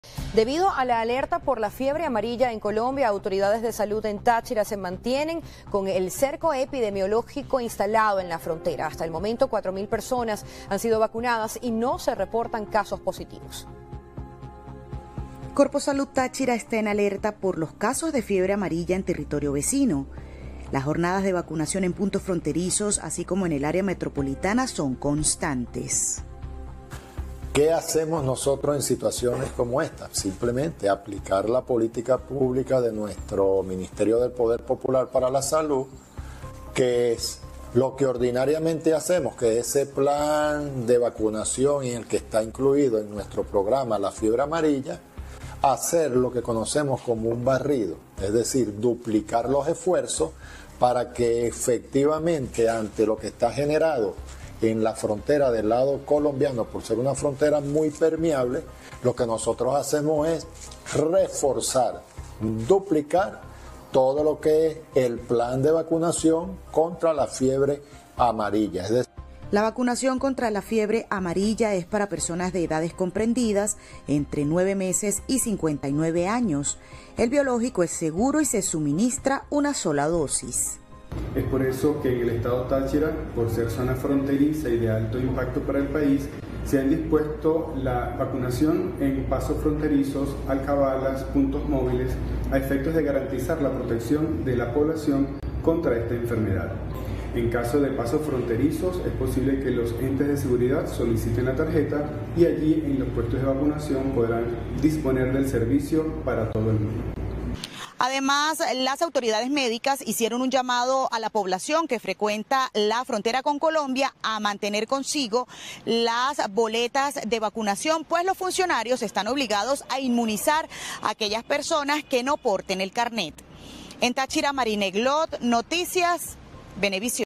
Autoridades de Táchira mantienen cerco epidemiológico en la frontera - 5 de diciembre de 2024
Debido a la alerta por fiebre amarilla en Colombia, autoridades de salud en Táchira se mantiene el cerco epidemiológico en la frontera. Hasta el momento cuatro mil personas han sido vacunadas y no se reportan casos positivos.